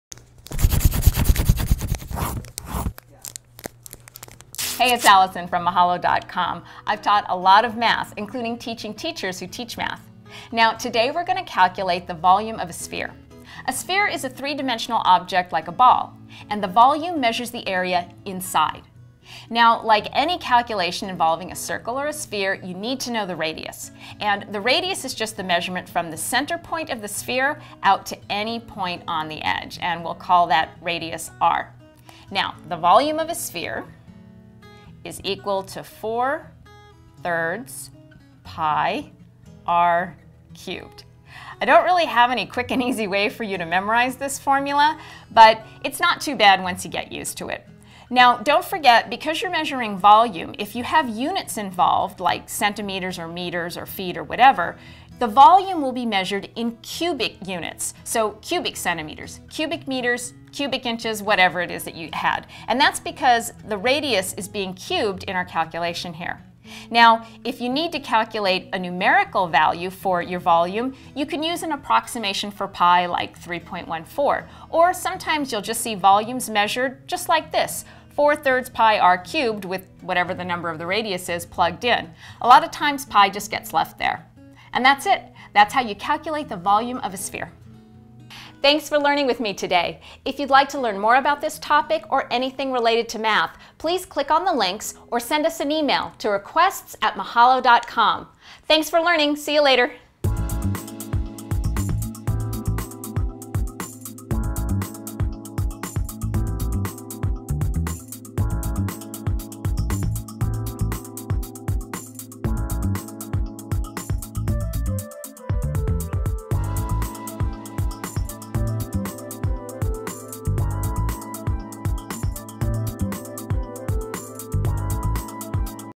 How to Calculate the Volume of a Sphere
Mahalo math expert Allison Moffett shows you how to calculate the volume of a sphere.

What Is a Sphere?

A sphere is the 3-dimensional version of a circle, like a ball or a marble. It is perfectly symmetrical; all points on the surface are exactly the same distance from the center (the measurement of this distance is the sphere's radius). It is also perfectly smooth; it has no flat edges or vertices (corners).cThe volume of a sphere is a geometrical measurement of the 3-dimensional space occupied by the sphere. For example, if you wanted to calculate the amount of water contained in a water balloon (assuming the water balloon were a perfect sphere), you would be looking for the water balloon's volume.c The formula used to calculate the volume of a sphere is V=(4/3)πr3.c

Step 1: Find the Radius of the Sphere

Before finding the volume of a sphere, it is helpful to know the values of different parts of the volume formula.  One part of the formula is the radius of the sphere.  Just as the radius of a circle is the distance from the midpoint of the circle to the outer edge, the radius of the sphere is the distance from the center of the sphere to any point on the sphere's surface.c This is denoted r³ and it is one-third of the formula for finding the volume of a cube.c

Step 2: Understand the Purpose of Pi

You now know two parts of the formula to find the volume of a sphere.  The volume of a sphere is actually derived from another formula, that being the volume of cylinders.  Mathematicians have proven that the two formulas are related and based on that, they found that the formula for finding the volume of a sphere is (4/3)πr³.c

Step 3: Use the Formula for Calculating the Volume of a Sphere

If you have a sphere with a radius of 6 inches, how much water can fill the sphere?

1. Set the problem up like this:  V = (4/3) x π x 6³
2. Find what 6³ is equal to: 6 x 6 x 6 = 36 x 6 = 216
3. Rewrite the formula with value for 6³: V = (4/3) x π x 216
4. Multiply 4 x 216 and divide the product by 3: (216 x 4)/3 = 864/3 = 288
5. Multiply the value you got in #4 by π: V = 288π.  Since the radius was measured in inches, the volume would be 288π cubic inches.

It is acceptable to leave the value as 288π since π is an infinite number.  If you want to multiply the value out, then you would take 288 x 3.14159, which equals 904.78 rounded to two decimal places.  The volume of water that can fill a sphere with a radius of six inches is 904.78 cubic inches.
c